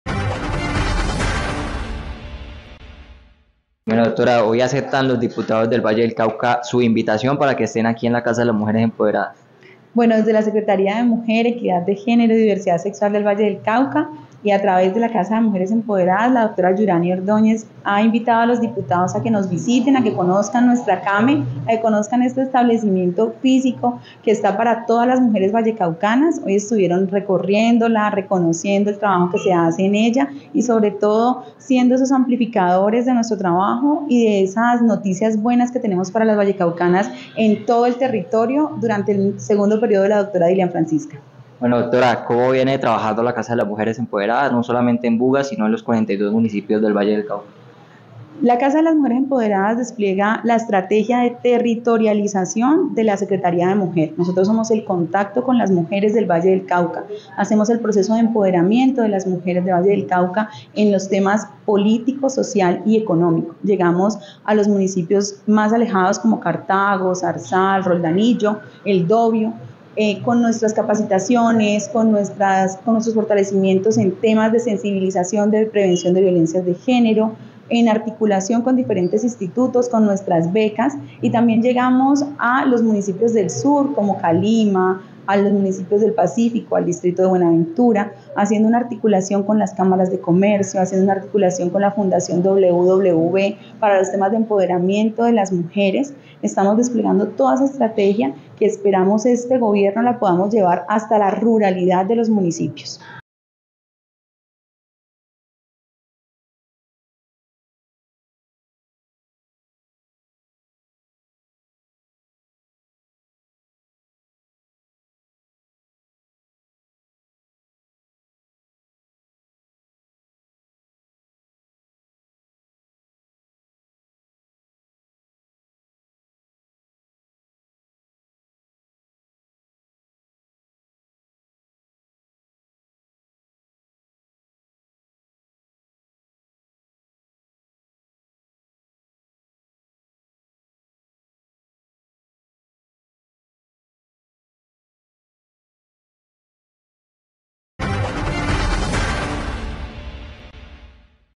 Diana Lorena Góngora Rivera Directora de la casa d
Diana Lorena Góngora Rivera Directora de la casa de la mujer empoderada nos habla sobre la visita de los diputados a la sede en Buga